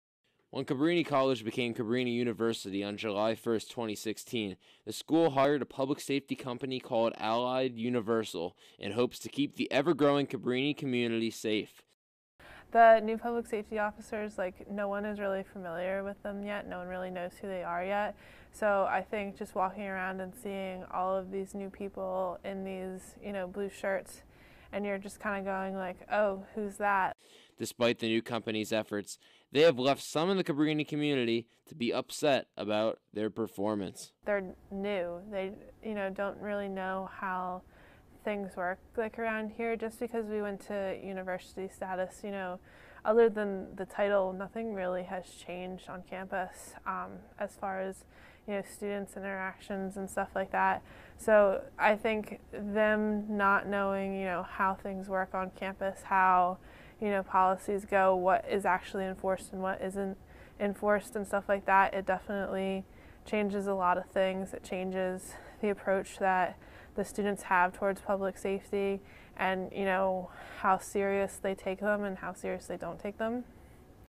Cabrini students upset with new Public Safety officers
Video by Emily Janny and John Williams.